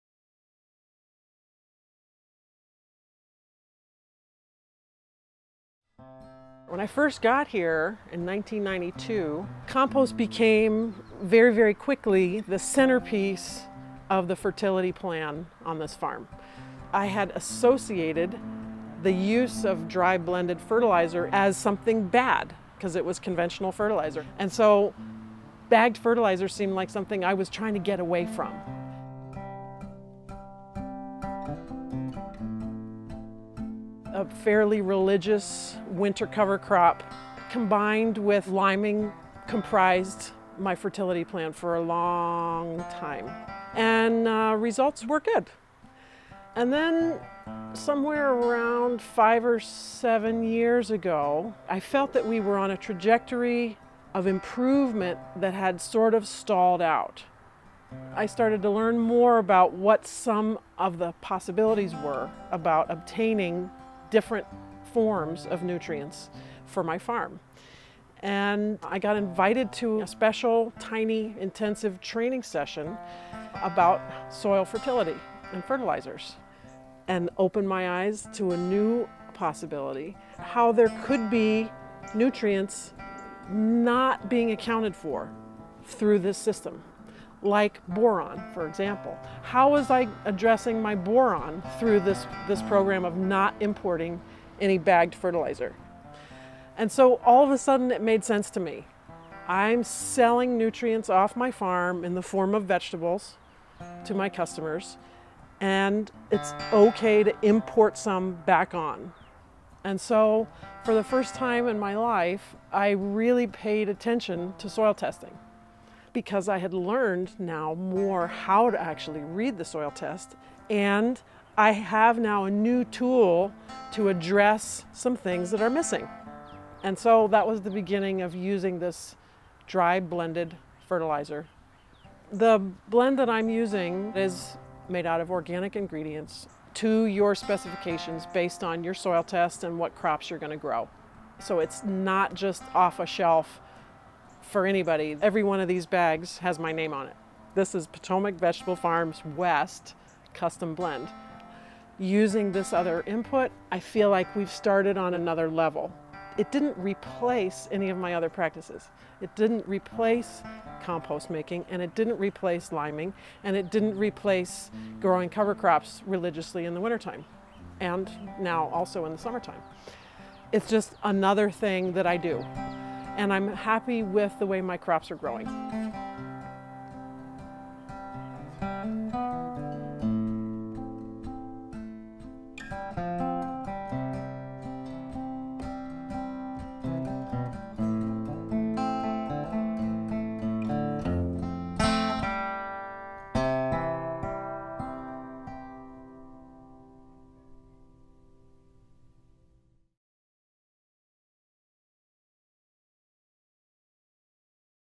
Fine-Tuning Fertility at Potomac Vegetable Farms
Ellen Polishuk’s strategy for managing soil fertility and plant nutrition has evolved over time. This video describes how this leading biological vegetable grower from Loudoun County, VA, now uses custom-formulated certified organic dry fertilizer to fine-tune fertility and complement her long-standing core practices of composting, liming, cover cropping, and crop rotation. Be sure to watch the associated profile video that provides an overview of Ellen’s overall soil management strategy, as well as the other technical clips with details about her soil-enhancing practices.